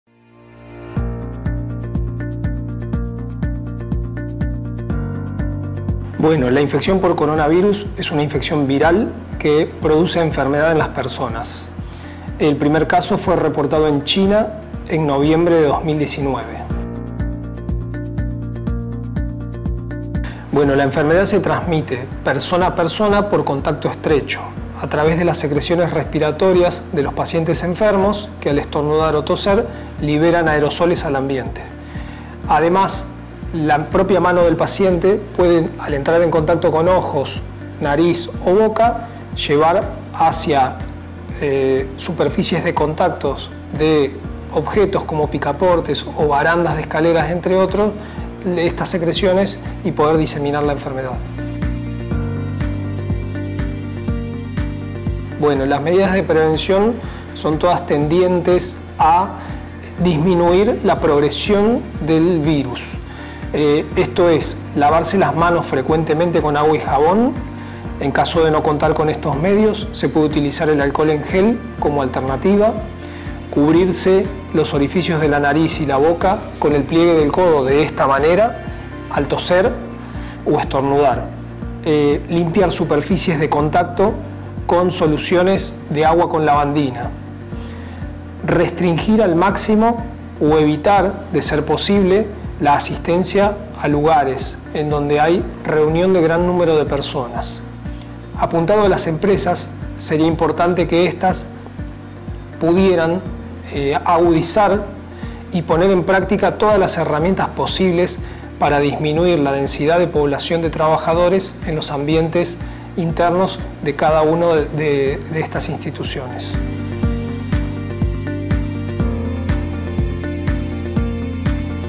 Coronavirus
AFA con Dr. Pablo Sosa sobre el Covid-19